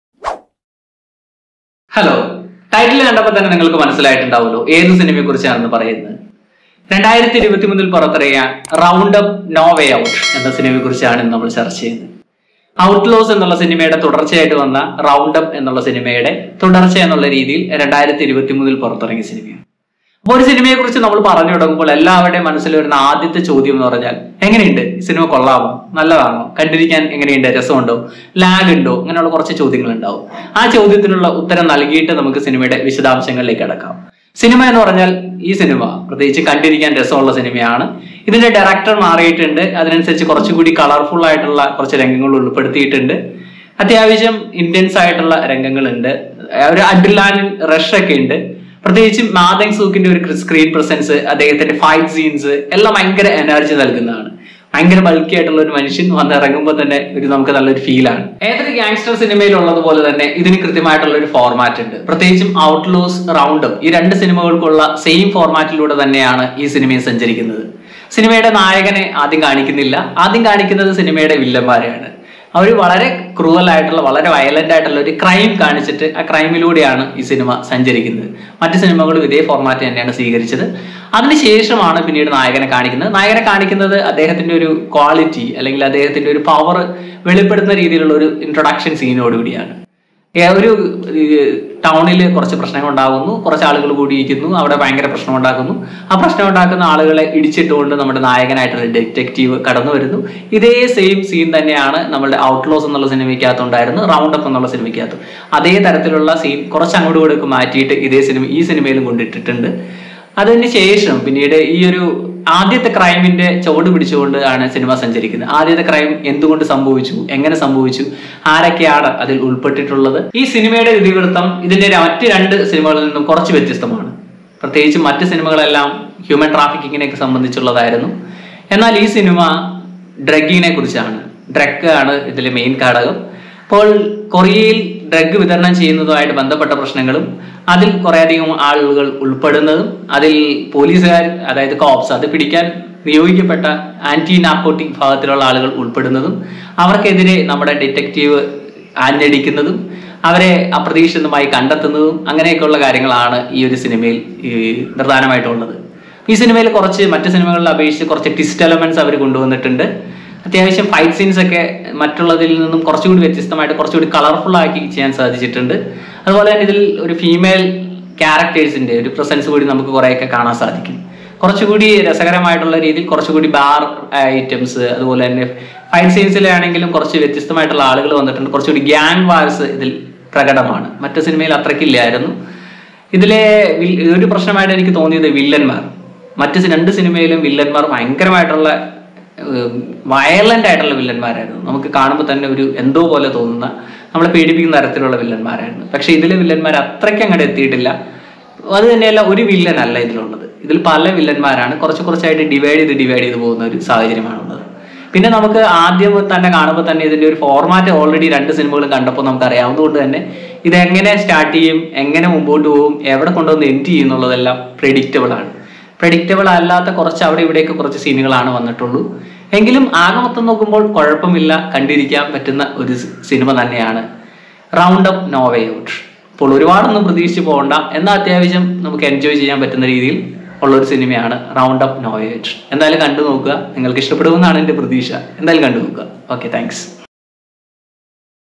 The Roundup No Way Out |Mlayalam Review| Korean action fim|Amazon Prime|Don lee| Violence
Seven years after the roundup in Vietnam, Ma Seok-do joins a new squad to investigate a murder case. He soon starts to dig deeper when he finds out the case involves a synthetic drug and a gang of thugs.